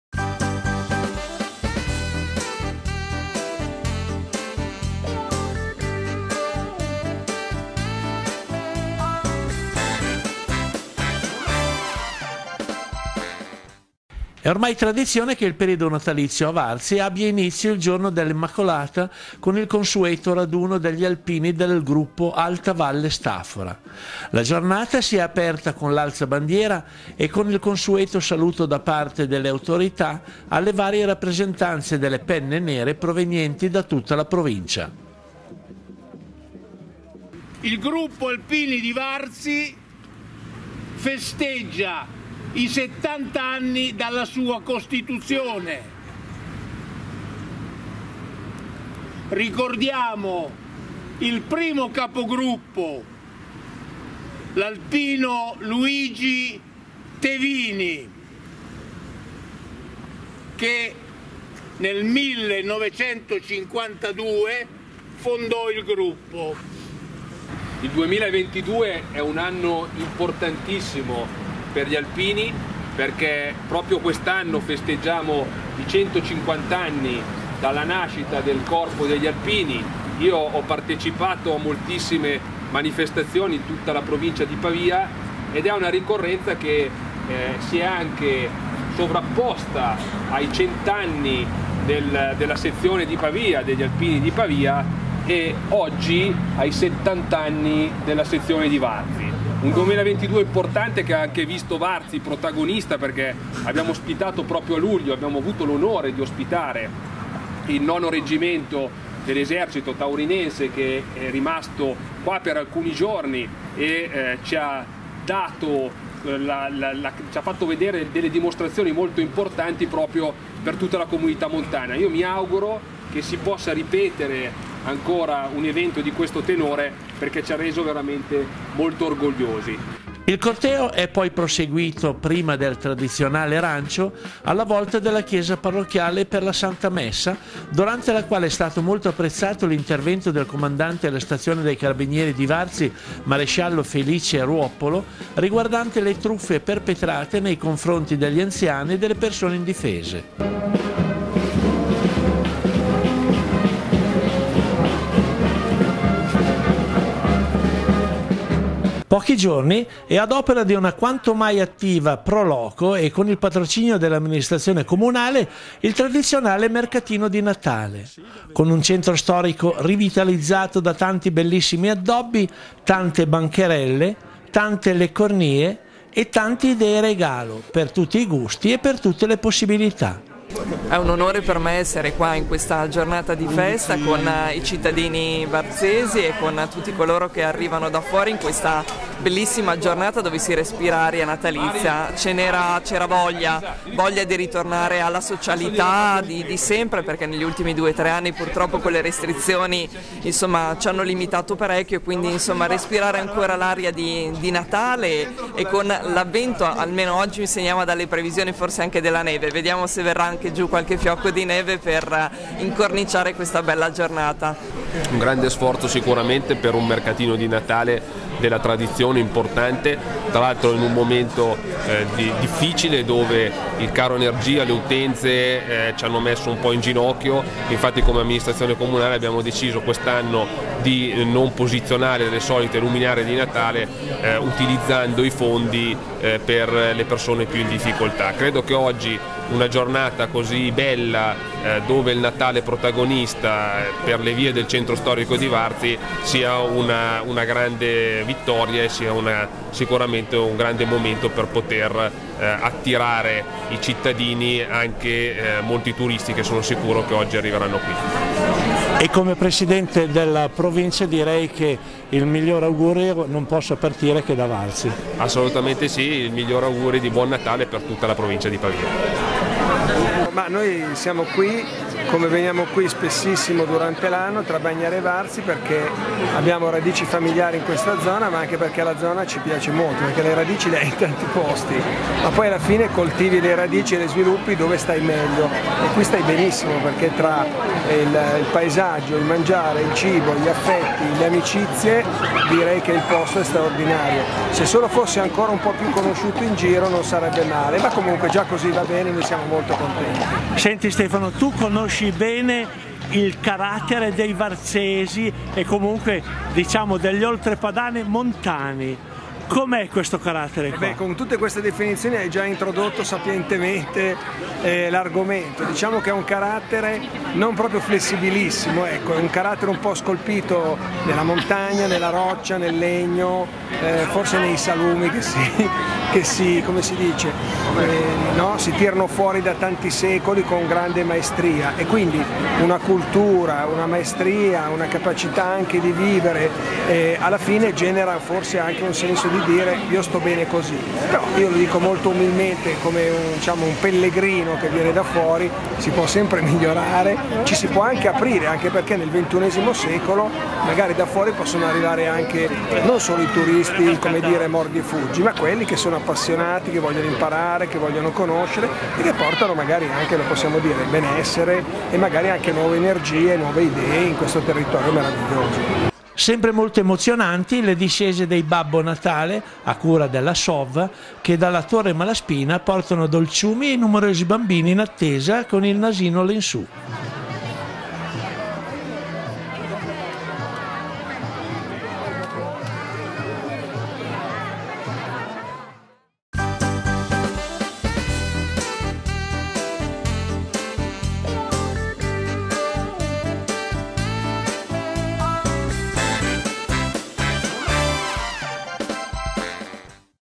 VARZI IN ATTESA DEL NATALE - DI REMO TAGLIANI
Da diversi anni il periodo natalizio a Varzi si apre con il raduno degli alpini del Gruppo Alta Valle Staffora seguito a pochi giorni dal tradizionale mercatino. In un unico servizio proponiamo i due avvenimenti che hanno attirato nel centro oltrepadano numerosi turisti.